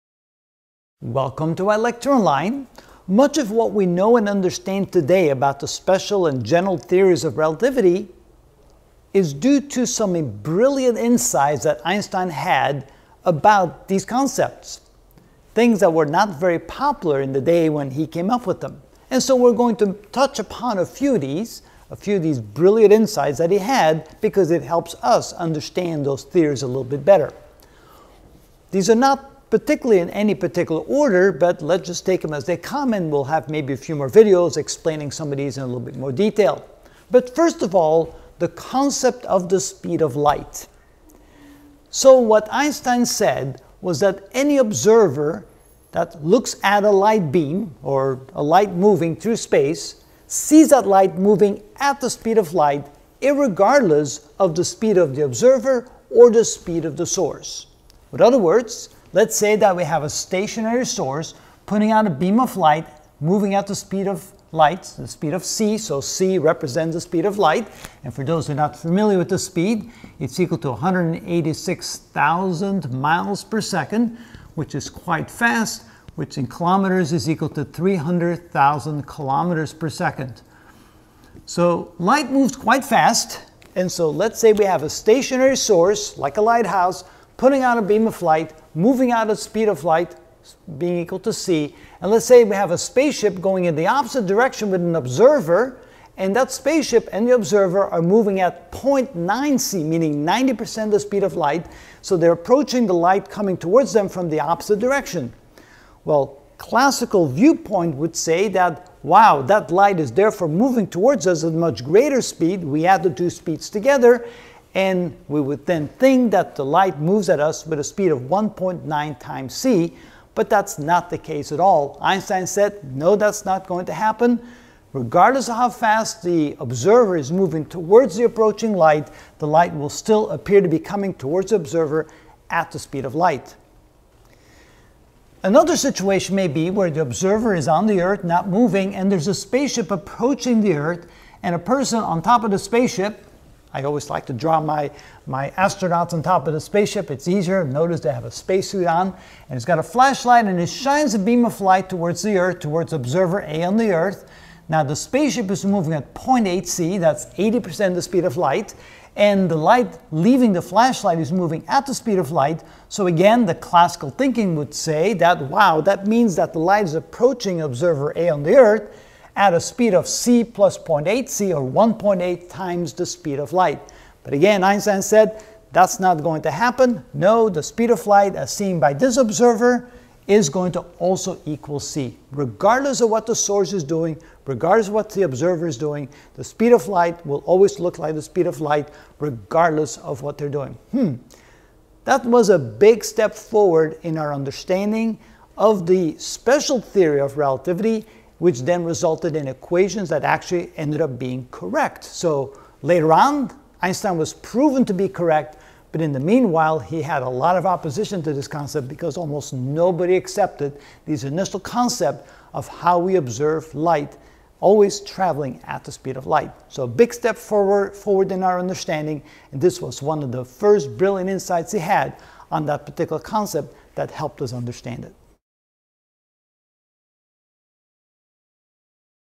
Physics 62.1 Understanding Space, Time & Relativity (4 of 55) Einstein's Brilliant Insight 1
In this video I will explain Einstein's brilliant concept that any observer sees light moving at the speed of light regardless of the speed of the observer or the speed of the source. An observer moving at v=0.9c towards a light source will see that light moving at the speed of light. Similarly, an observer (stationary, v=0) on Earth will see light moving at the speed of light from a space ship traveling towards Earth at v=0.8c.